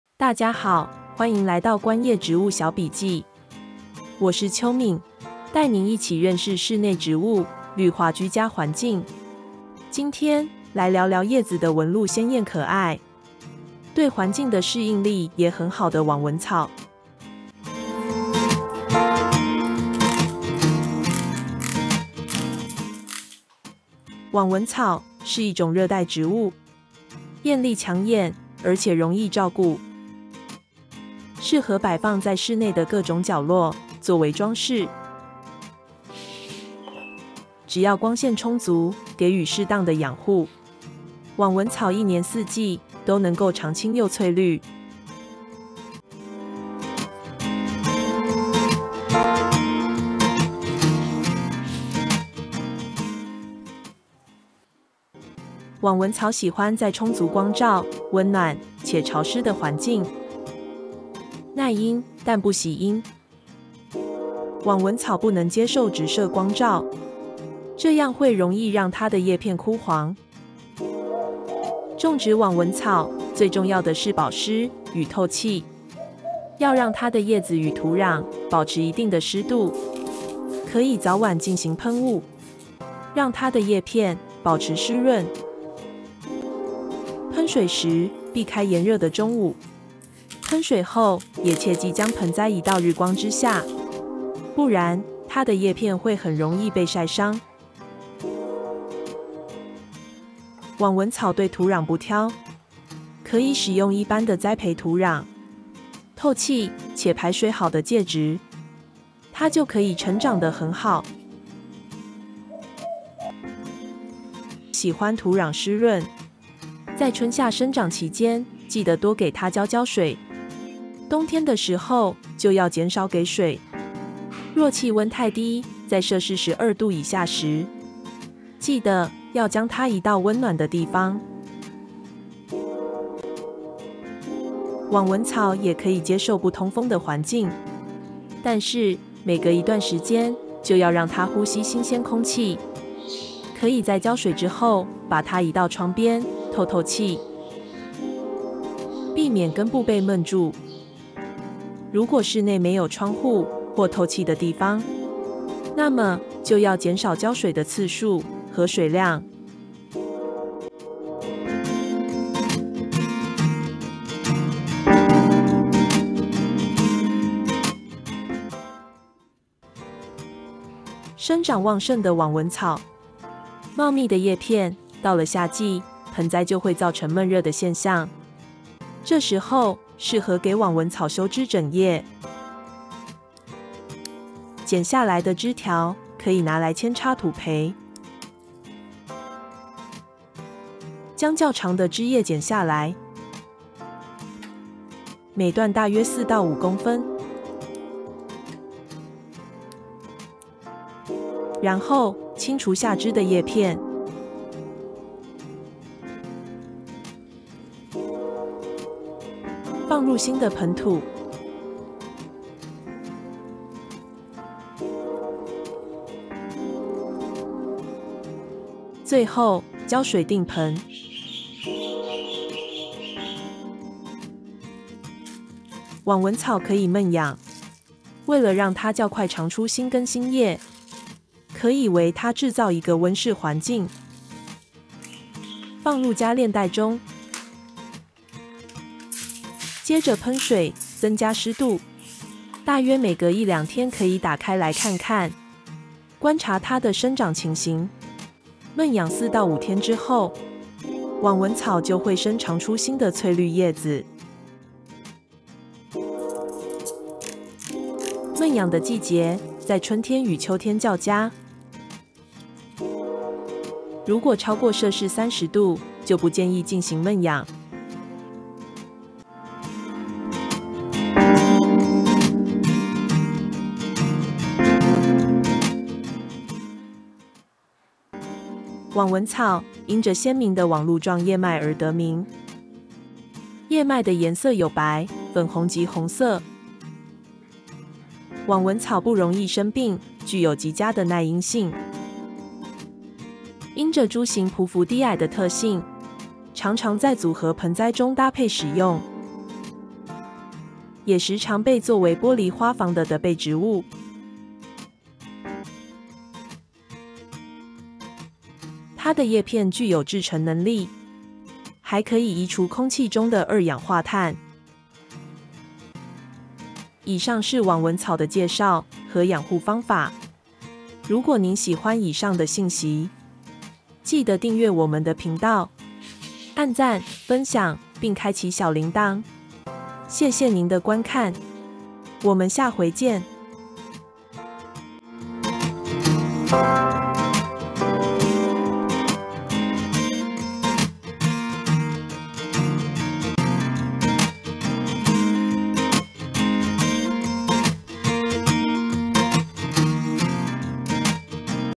網紋草如何養護、扦插繁殖及悶養方法 Fittonia Plant care, cutting propagation and greenhouse
網紋草時常作為室內盆栽，
也可以在玻璃花房中看見網紋草作為地被植物。
是一種很好養護的觀葉植物，
放在室內具有極佳的醒色作用。
影片中的品種是紅焰網紋草，以及白天使網紋草。
養護方法、扦插繁殖，土培，以及悶養...等等注意事項
希望您會喜歡！

Fittonia Plant is often grown as a houseplant, and it can also be seen as a ground cover in glass greenhouses.
It is a well-maintained foliage plant, and it has an excellent wake-up effect when placed indoors.
The Fittonia Plan in the film is available in red and white.
Fittonia Plant care, cutting propagation, soil cultivation...etc.

00:00 網紋草介紹 Fittonia Plant
00:47 如何照顧網紋草 Fittonia Plant care
02:42 扦插土培，以及悶養 cutting propagation & greenhouse
04:28 綠化居家及淨化空氣 home greening & purifying the air

✔ You can choose from a variety of subtitles.
🗣 The narration of this video uses AI voice.